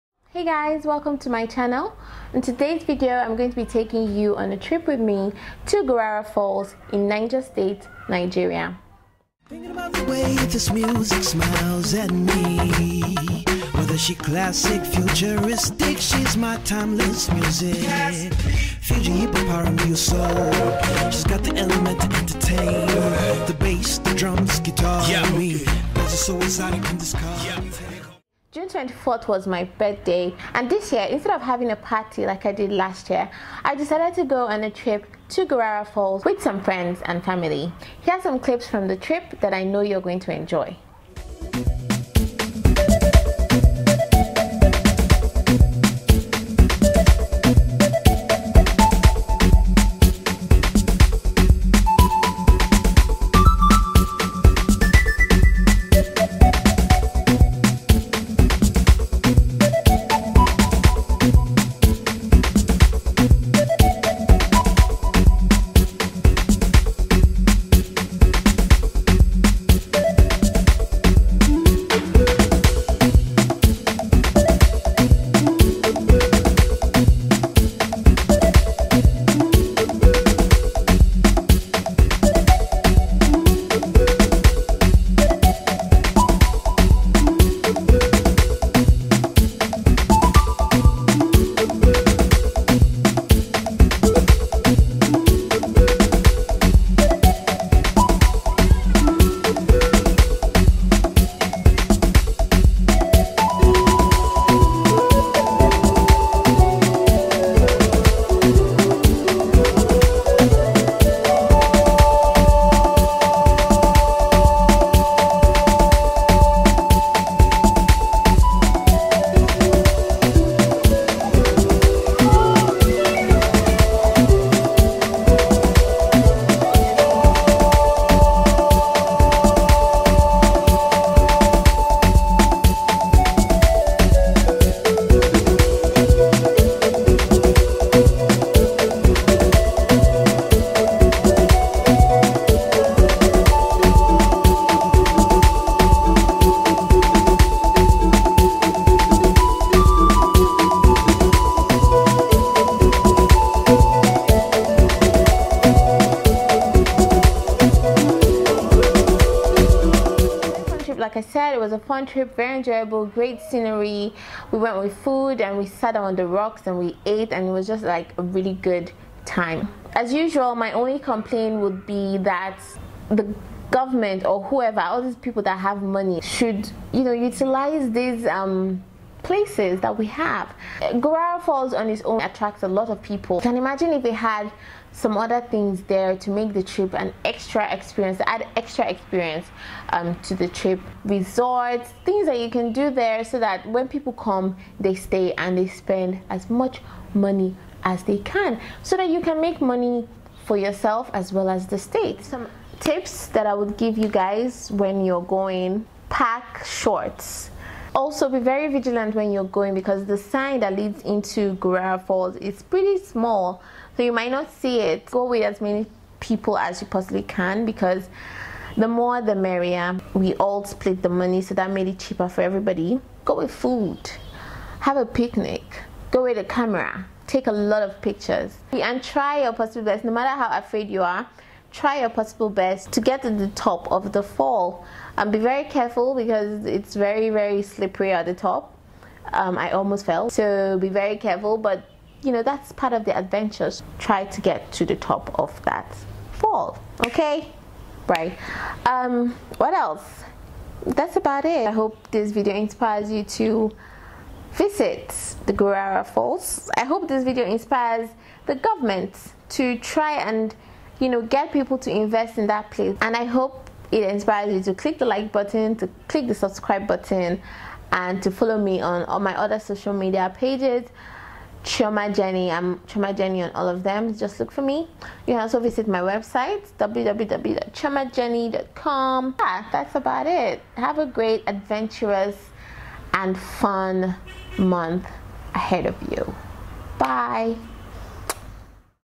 ROAD TRIP TO GURARA FALLS | CHIOMAJENNI
Hey loves,

I finally found my way to Gurara falls!! Yey, I have been meaning to visit for a while now and my birthday this year gave me the push I needed to make the trip. It was such a last minute planned trip but I am glad I did it and appreciate all those who came out with me.

WATCH THE VIDEO FOR TIPS AND CLIPS ABOUT THE TRIP :)